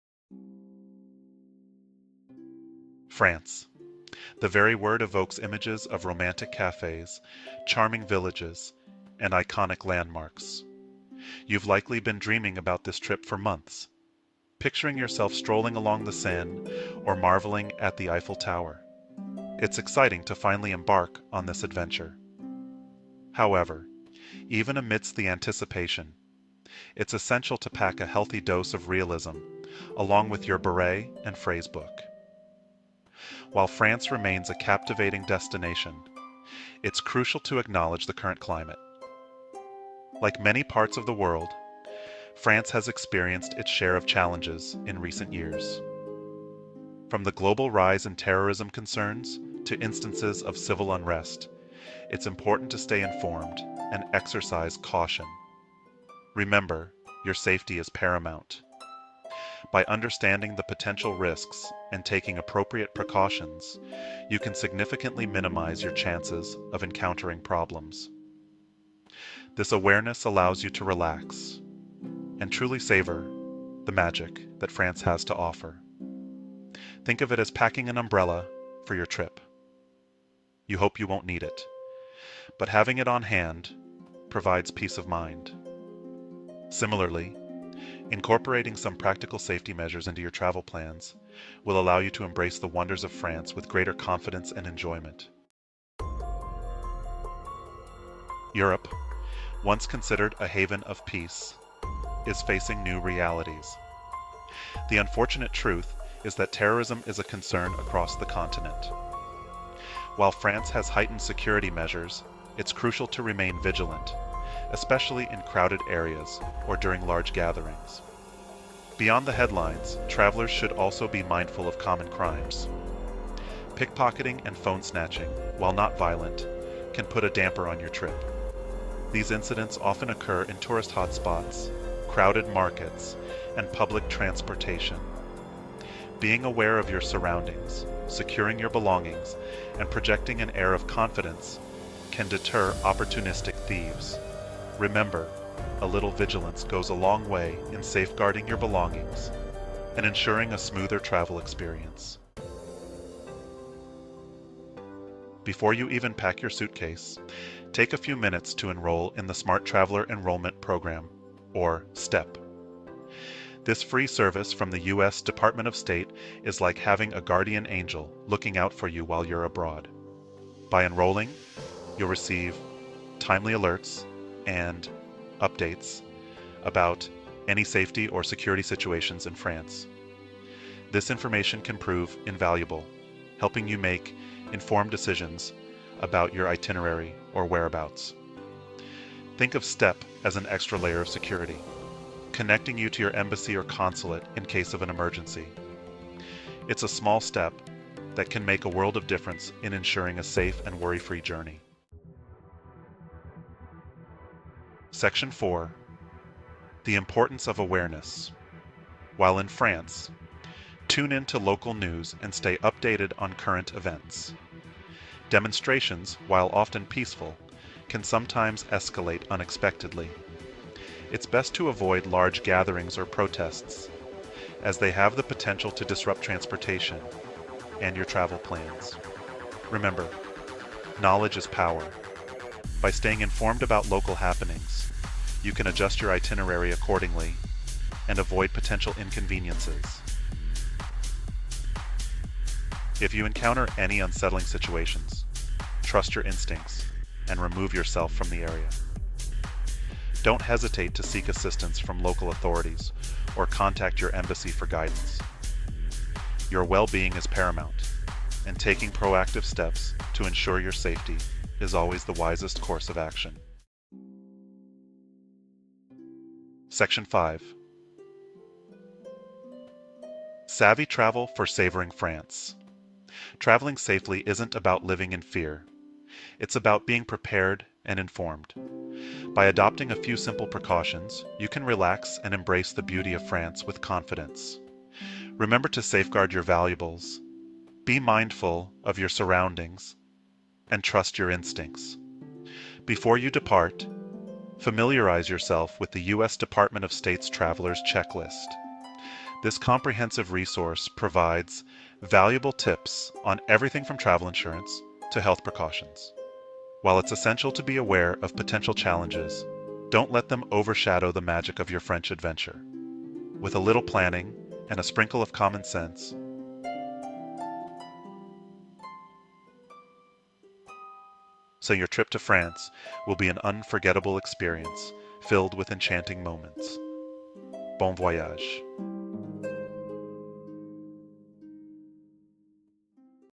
France: ‘Exercise Caution’ or ‘Bon Voyage’? (Official Video)
Terrorist groups continue plotting possible attacks in France. Terrorists may attack with little or no warning, targeting tourist locations, transportation hubs, markets/shopping malls, local government facilities, hotels, clubs, restaurants, places of worship, parks, major sporting and cultural events, educational institutions, airports, and other public areas.

Incidents such as pickpocketing and phone snatchings occur frequently and can happen anywhere, especially in crowded areas such as airports, train stations, subway and train cars, and near tourist attractions.

Peaceful demonstrations and strikes in Paris and other cities throughout France occur regularly and can disrupt transportation. On rare occasions, demonstrations have included violence and property damage and police have responded with water cannons and tear gas.

OUTLINE:
00:00:00 A Dream Trip, With a Dose of Reality
00:01:35 The Reality of Today's Europe
00:02:32 Smart Traveler Enrollment Program (STEP)
00:03:21 The Importance of Awareness
00:04:23 Savvy Travel for Savoring France